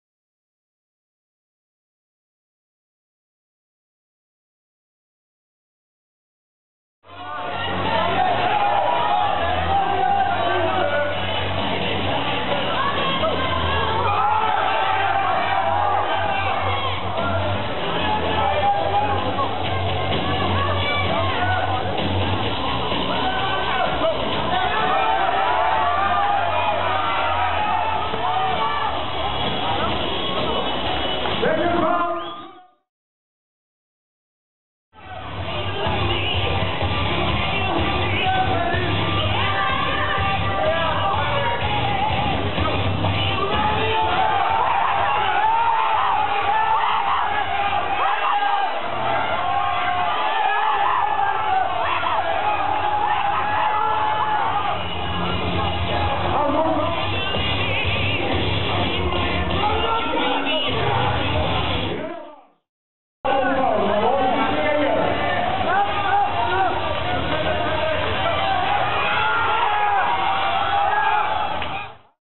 Swedish Nationals 2010 Men -70 Left
Swedish Nationals 2010 Men -70 Left Marcus Ärlebrandt vs Freddy Gunnarsson